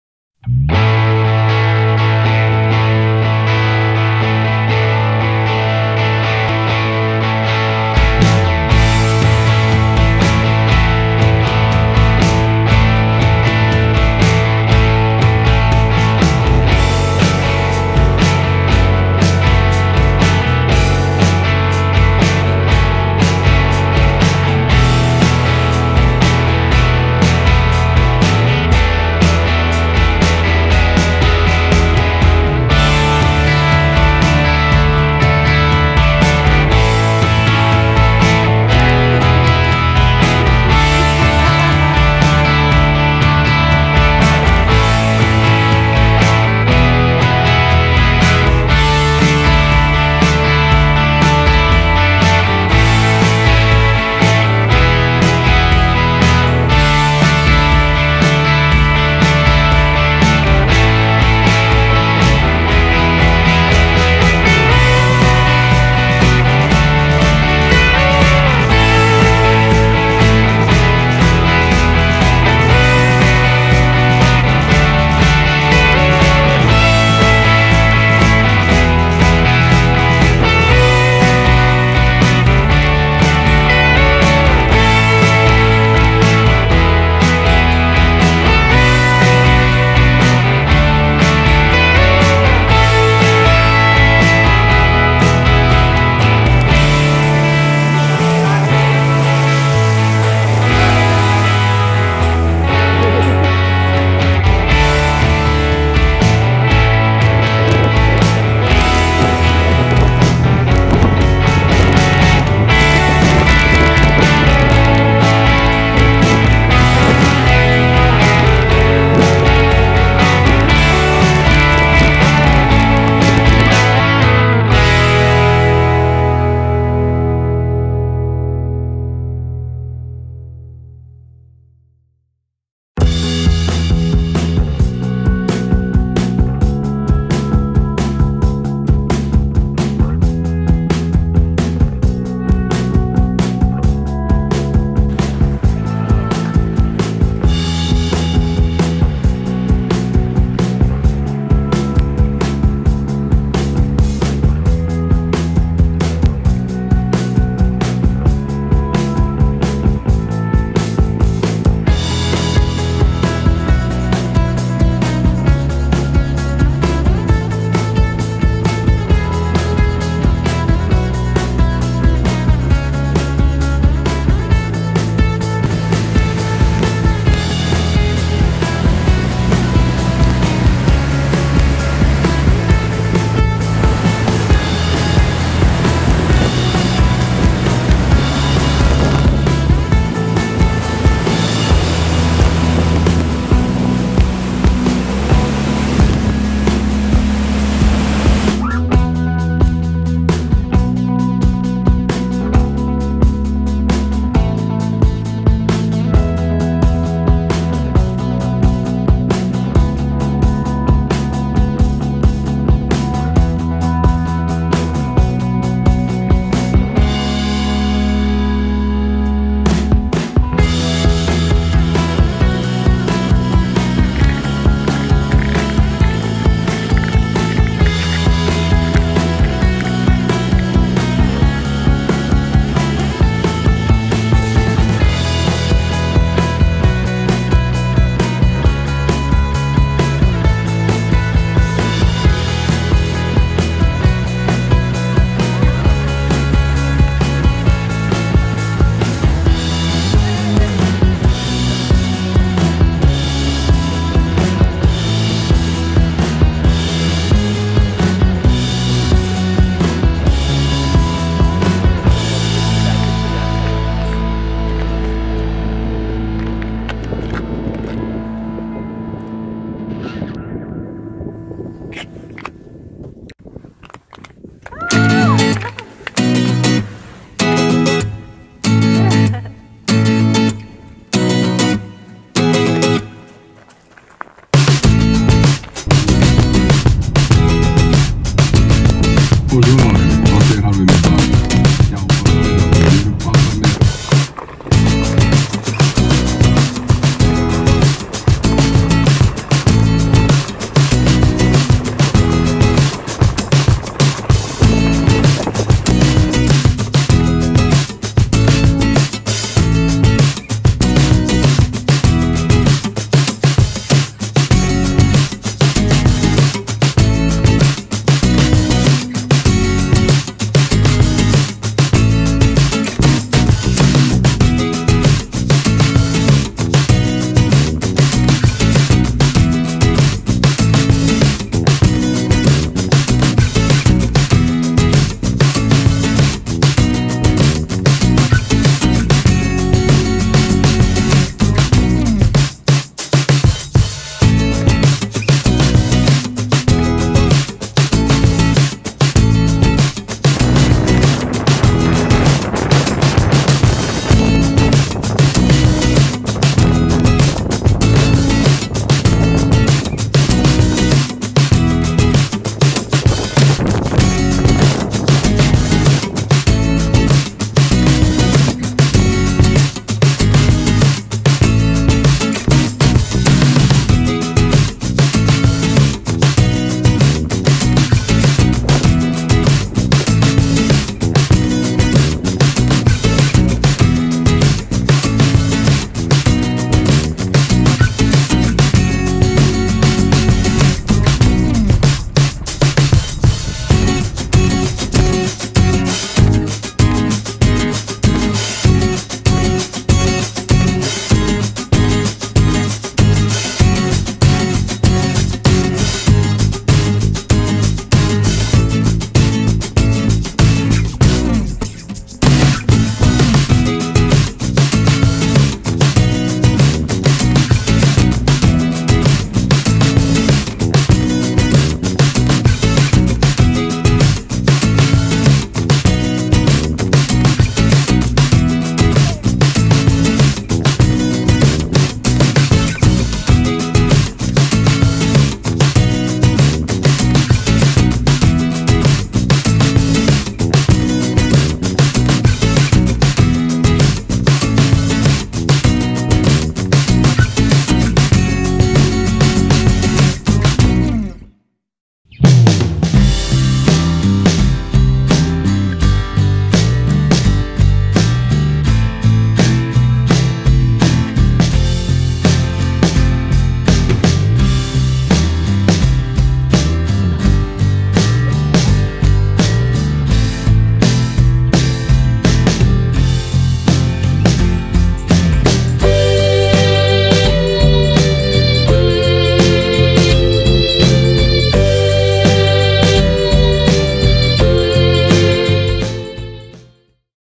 South Iceland Camper Trip - CamperStories
This is how to explore South Iceland in a Camper van. This couple did it in seven days and loved it. This video is a part of their CamperStories entry. You can read their full story here:
Happy Camping!